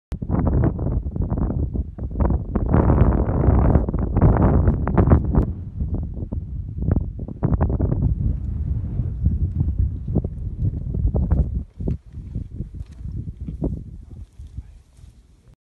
Ben A’an Lock Katrine ( Scotland )
Blue sky in Scotland as contemplating a blue lake... I have noticed a desert kind of look also, which is rare for this geographical region.